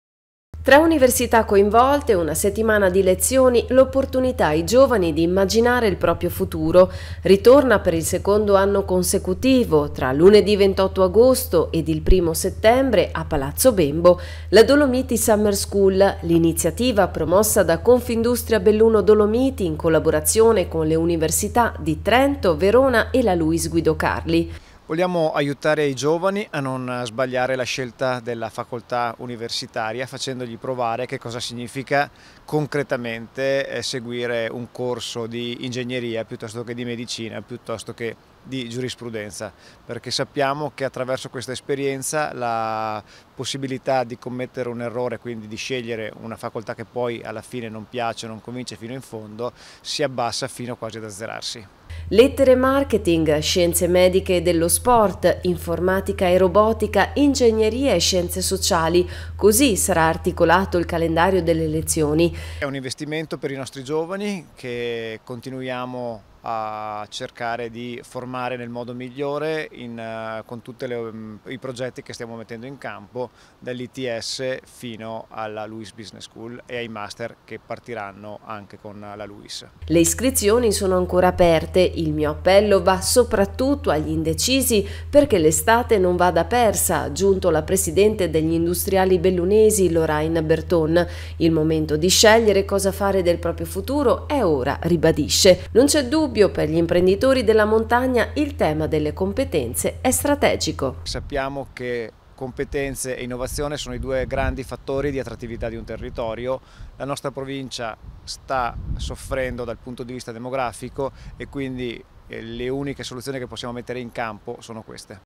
Ritorna la Dolomiti Summer School, per Confindustria Belluno strategico il tema delle competenze
Si rinnova la collaborazione fra gli industriali bellunesi e le università di Trento, Verona e la Luiss Guido Carli in ottica di orientamento universitario per i giovani bellunesi, con il fine di agevolare poi il futuro inserimento lavorativo.